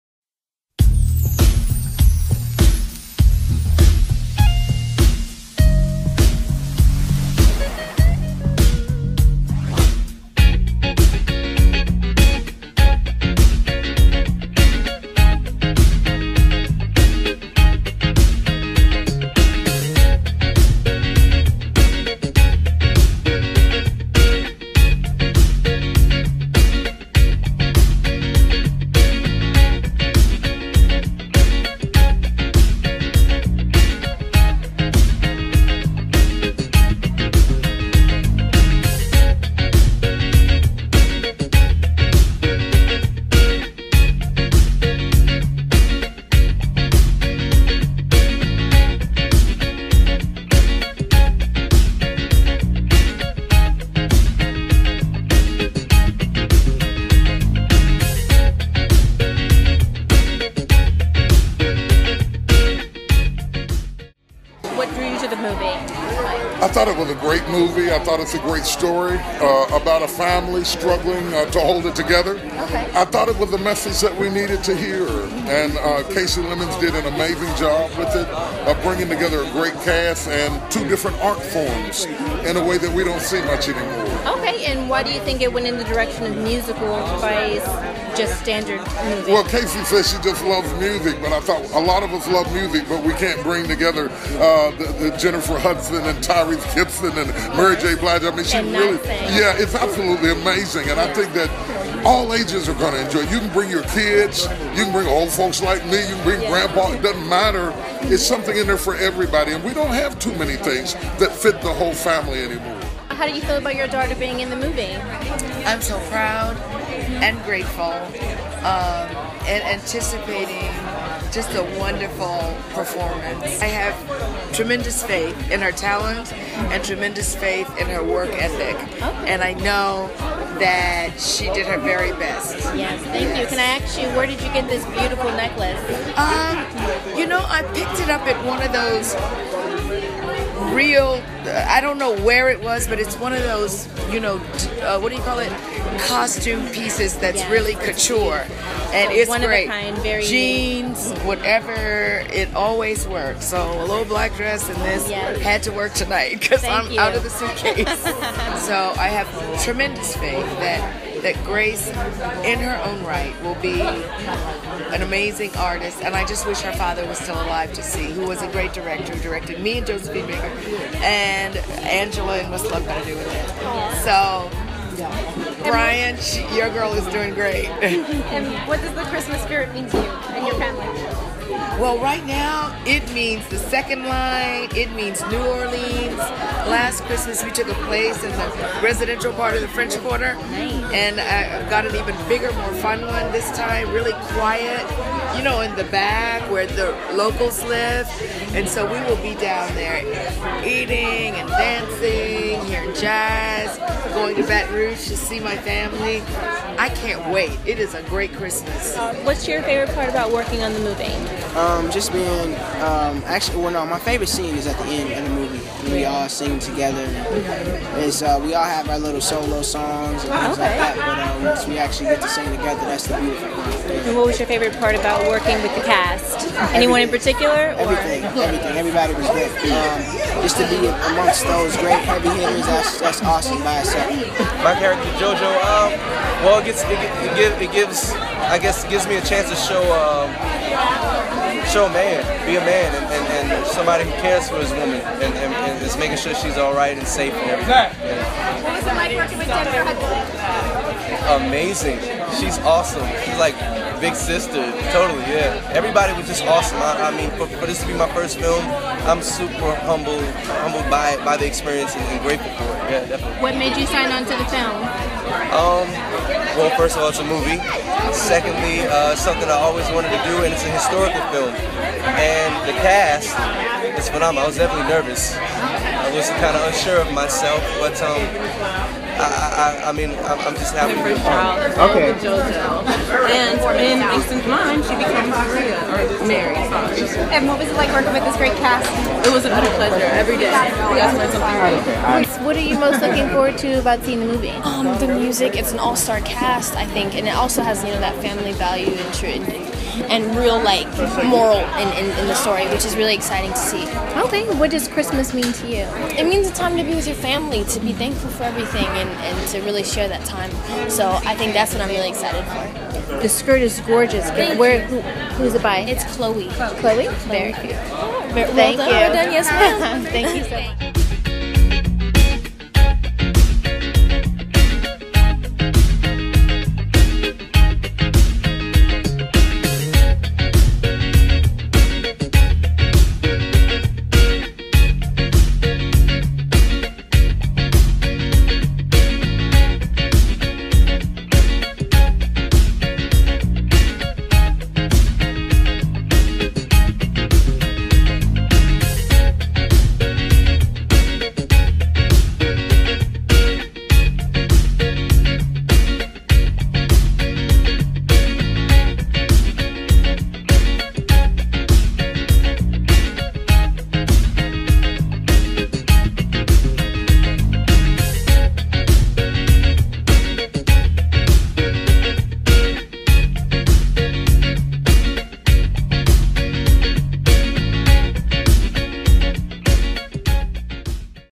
Black Nativity Screening Premier
The world famous Apollo Theatre played host for the movie premier of the adaptation of Langston Hughes original play "Black Nativity", starring Forest Whitaker, Angela Bassett, Jennifer Hudson, Mary J. Blige and Tyrese Gibson. The Kasi Lemmons directed musical holiday season movie will make for a great family night out.